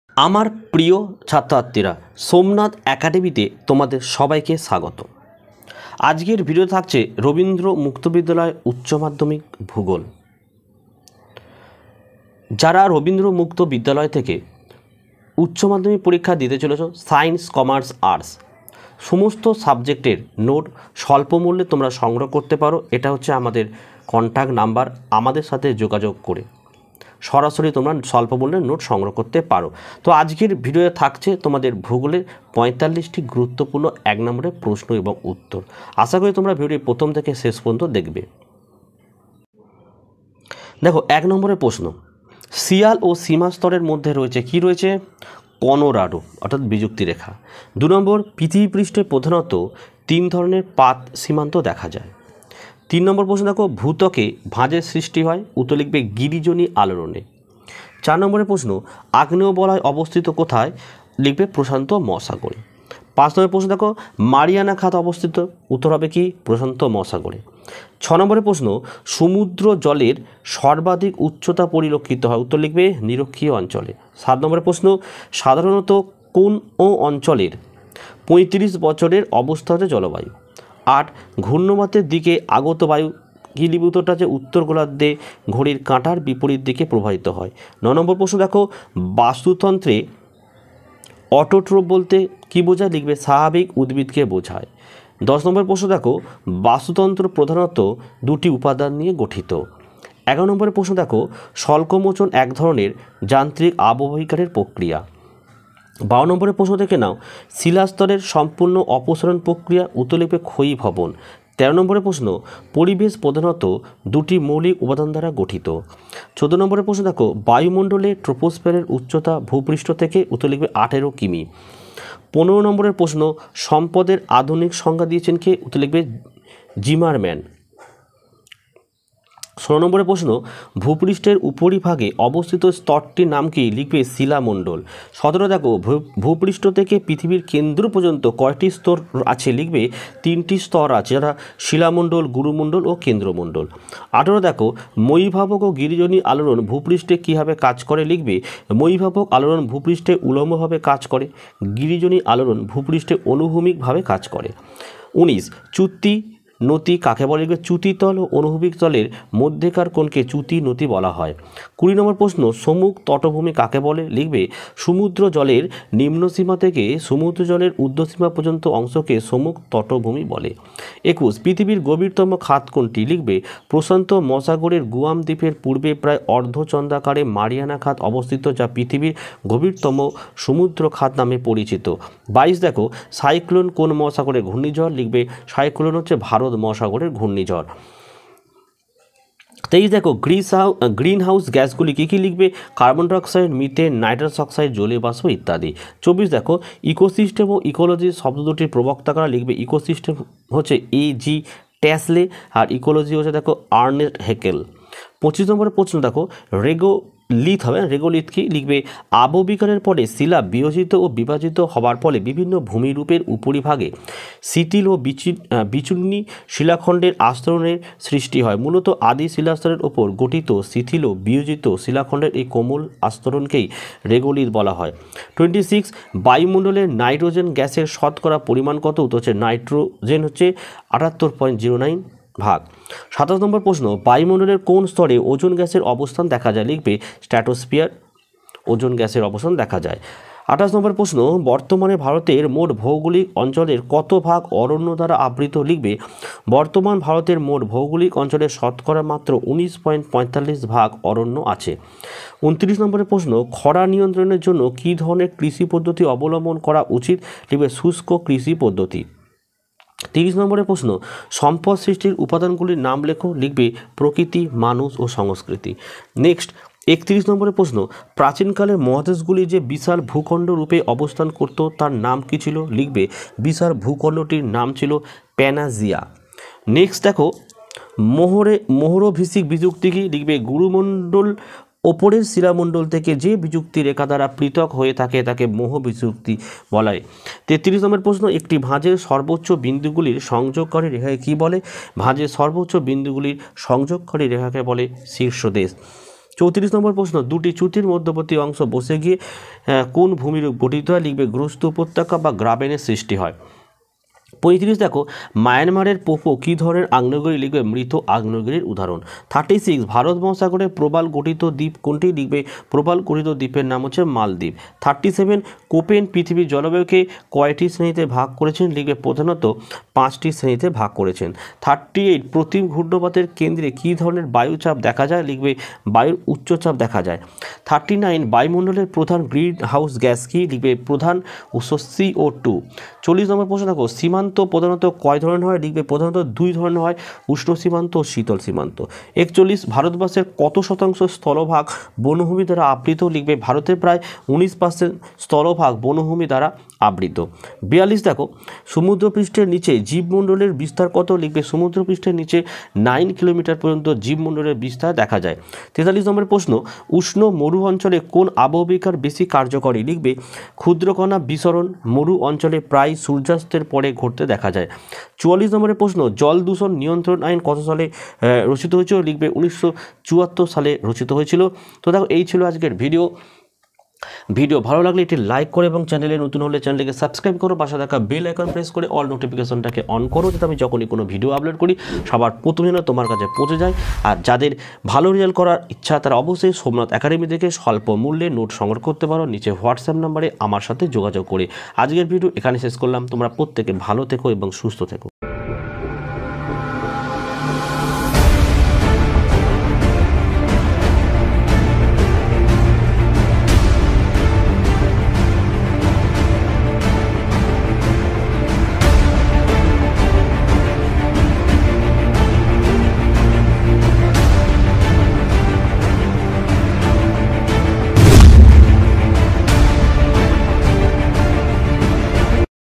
Rabindra Mukto vidyalaya HS 2024 Geography 1 mark question Suggestion with answer
Hello students you are watching somnath Academy YouTube channel. In this video lessons we will discuss Rabindra Mukto vidyalaya HS 2024 geography Suggestion